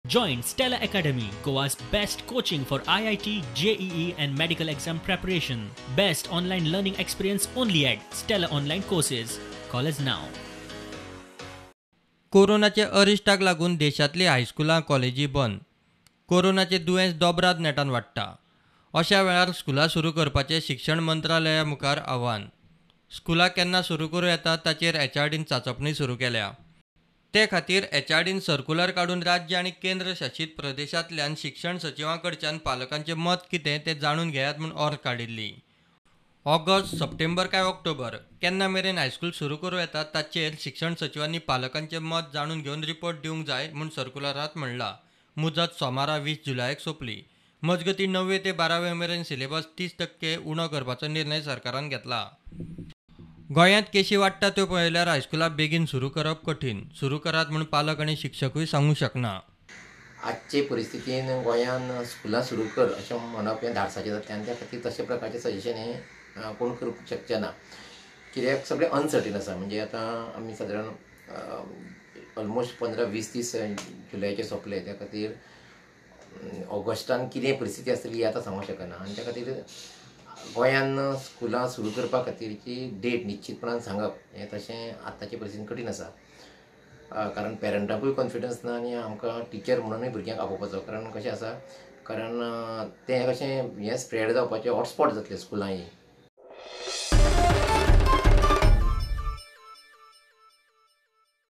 HRD SEEKS PARENTS' OPINION ON WHEN TO REOPEN SCHOOLS_Prudent Media Goa
As educational institutions have been shut due to covid19 pandemic, the Ministry of Human Resource Development has now sought parents' opinions on when to reopen schools this year. HRD has written to education secretaries of all states, and have directed them to get feedback of parents on when they would want schools to reopen and changes they expect to be introduced in schools.
HRD has asked secretaries to ascertain if will be comfortable with reopening schools in August, September or October 2020, and what are the parents' expectations from schools as and when they re-open. The ministry has asked the states to file their responses by 20 July